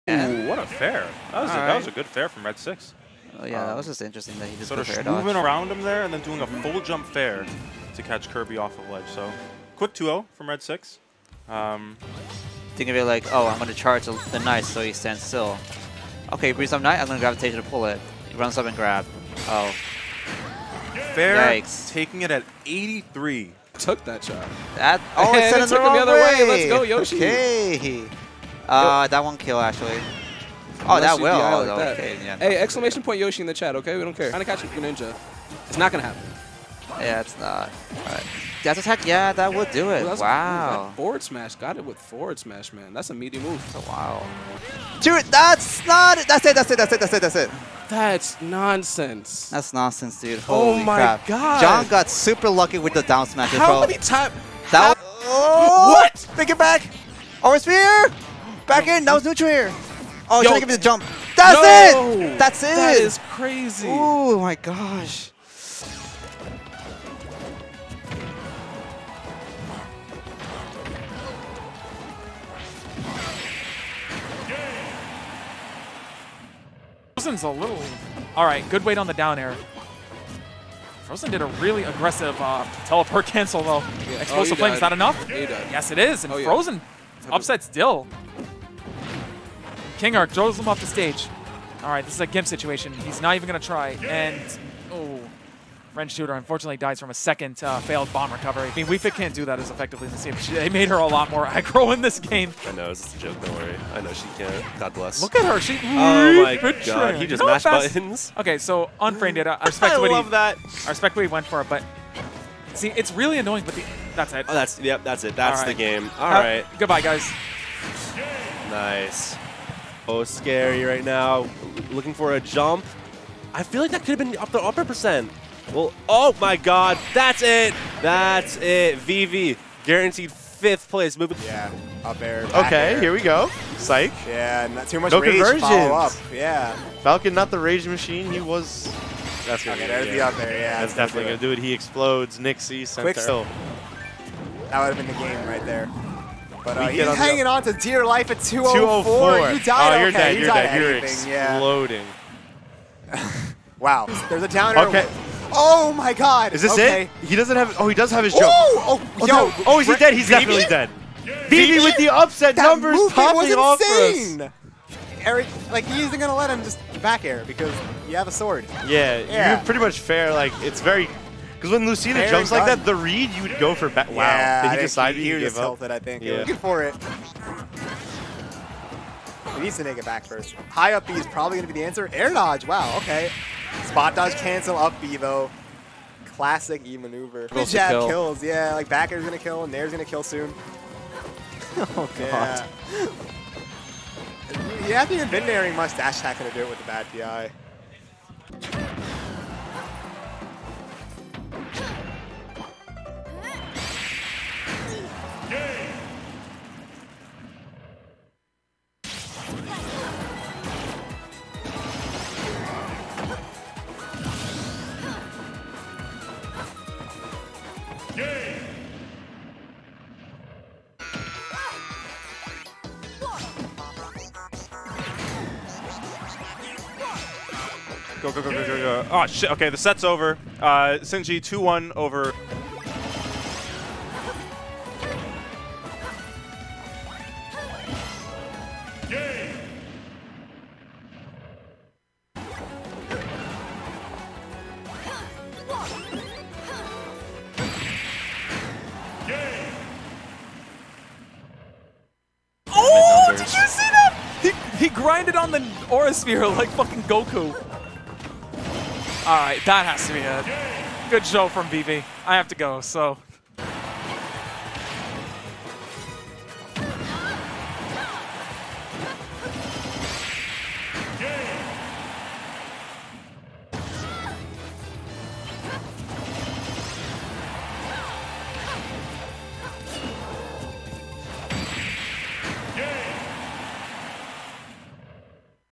Xeno165 - Finishers
Demise Mr.E(Lucina White Black) vs Ho3K Gen(Palutena red)

 - Xeno165
 - 62 Orchard St 2nd floor, New York, NY 10002, USA

Commentators:
 - Ho3K John Numbers (@JohnNumbers)

Venue Fee: $8
Singles:   $7
Doubles:   $5

Venue Address -
62 Orchard Street 2nd Floor